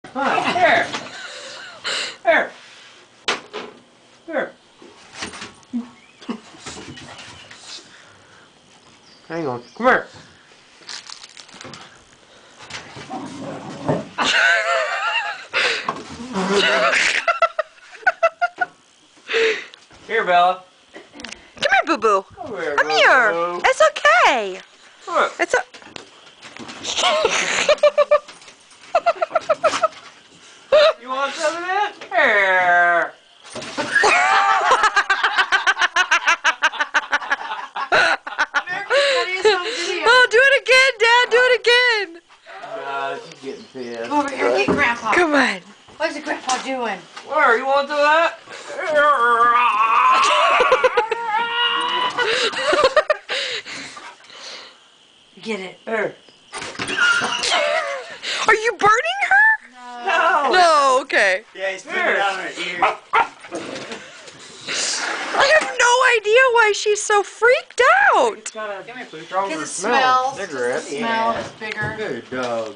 Crazy Dog
dog hates the smell of cigar